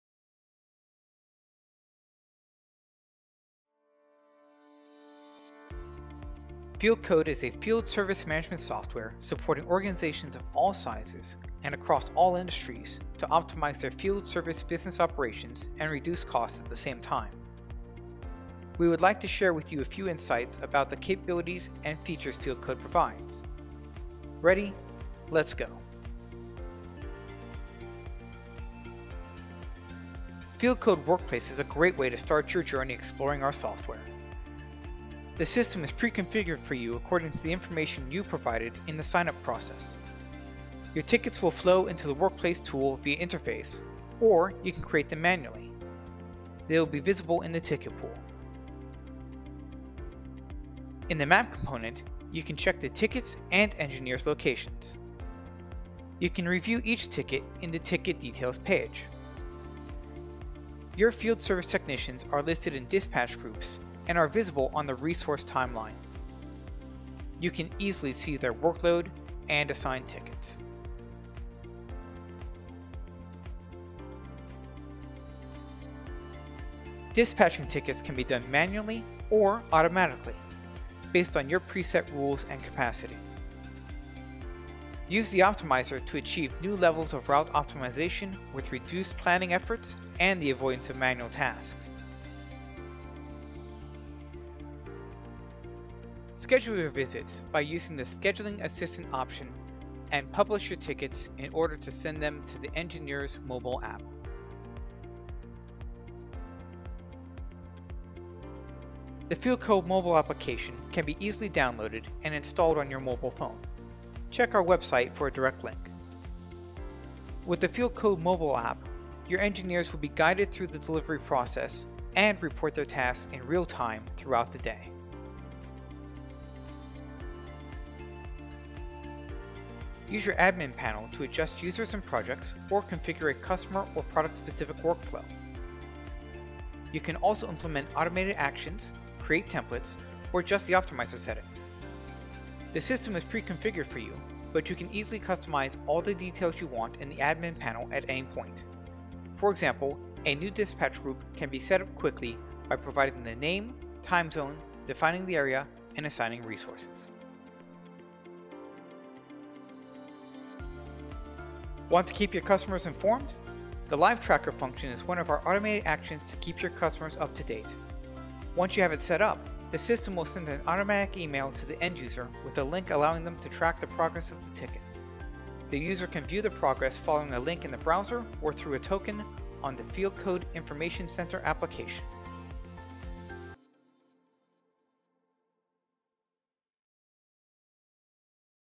Fieldcode product demo: Understanding the core features of our software
Fieldcode is the #1 Field Service Management software without fixed cost supporting organizations of all sizes and across all industries. It operates on a pay-per-event model which allows for significant cost reduction while promoting climate positivity. In this video, we are sharing insights about the capabilities and features that Fieldcode provides. Manage end-to-end operations in a field service lifecycle from ticket creation, route scheduling, dispatching to analytics and invoicing efficiently. Access the software instantly in three easy steps